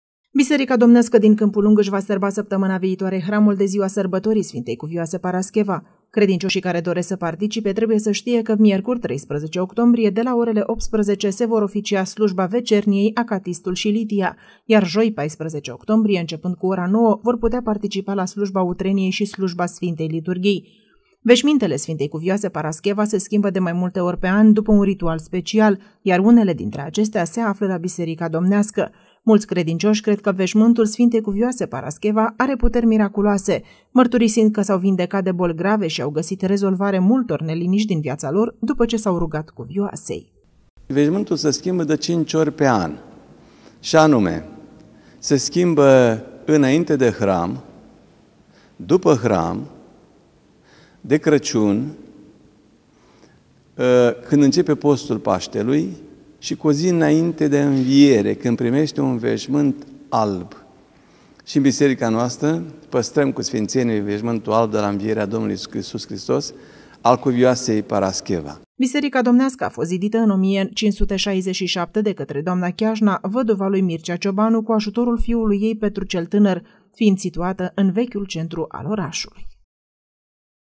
Jurnal MUSCEL TV 08.10.2021 Hram la Biserica Domnească de Sf. Parascheva din Câmpulung
Moaștele Sfintei Cuvioase Parascheva au ajuns la noi în țară în anul 1641. Acestea au fost mutate în Catedrala Mitropolitană din Iași în anul 1889, unde se află și astăzi. Creștinii pot merge la Biserica Domnească ce adăpostește veșminte ale Sfintei, lăcaș de cult care își va serba și hramul săptămâna viitoare.